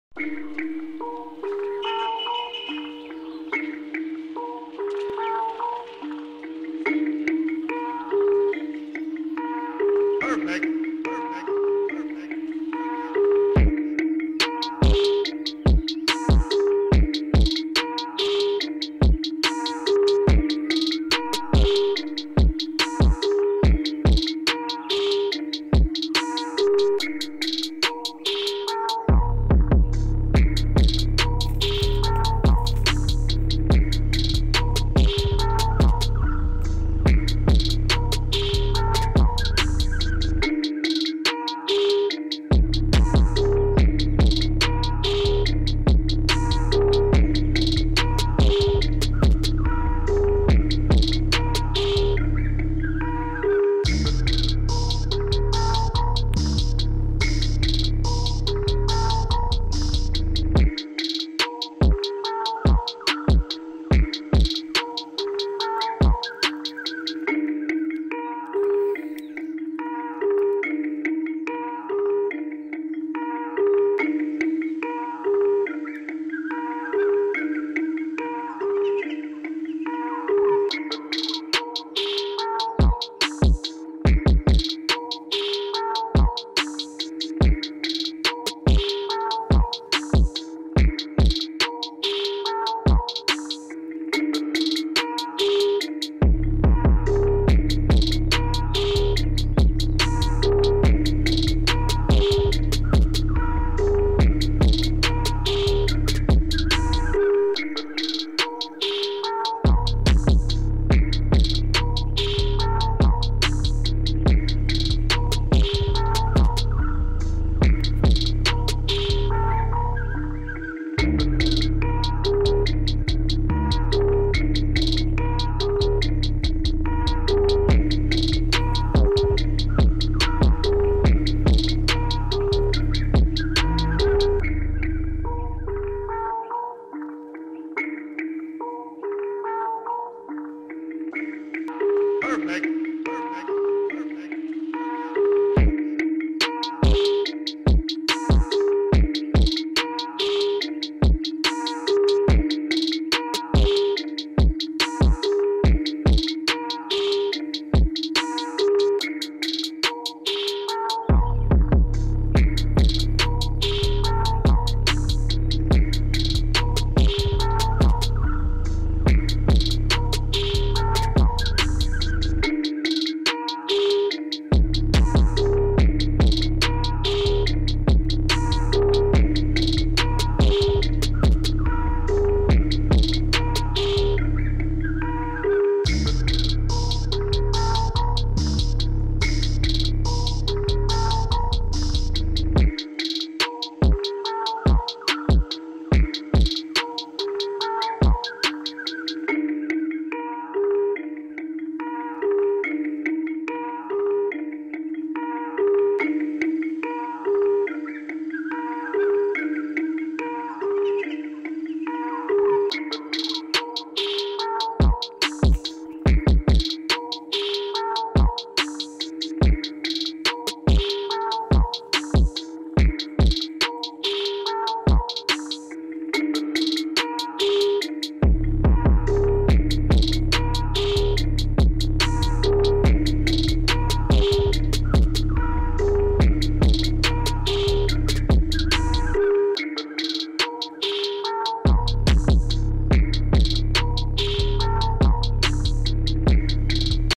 Amazing scene when peacock peaks in the morning | Indian WildLife
A scene when peacock peaks in the morning